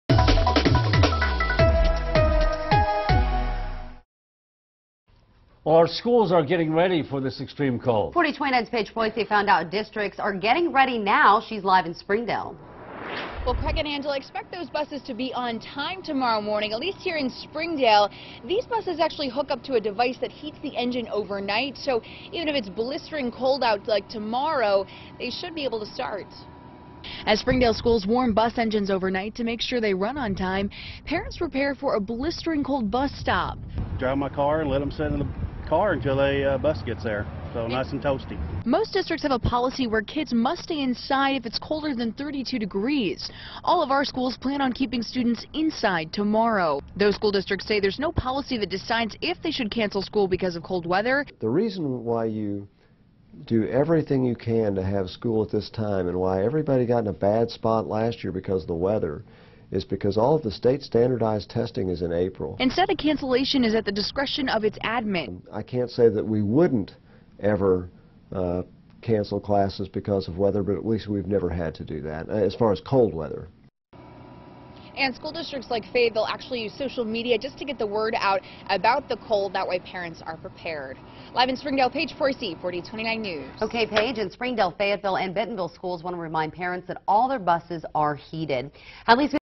School districts get ready for arctic blast
Springdale schools has technology to help school buses to start despite the bitter cold.